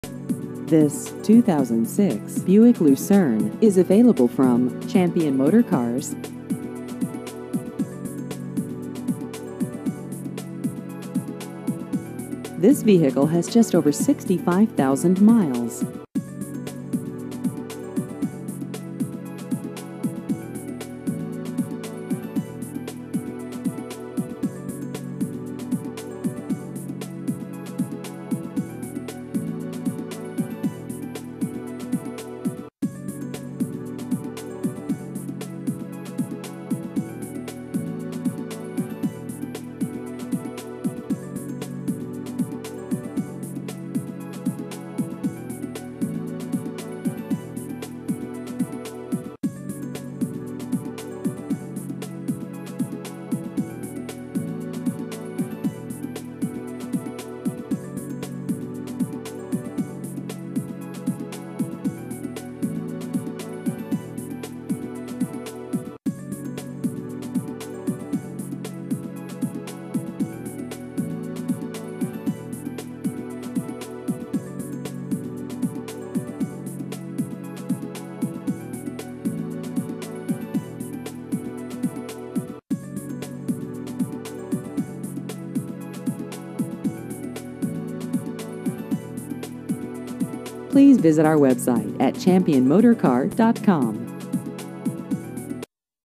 2006 Buick Lucerne Used Cars Machesney Park IL
This 2006 Buick Lucerne is available from Champion Motor Cars.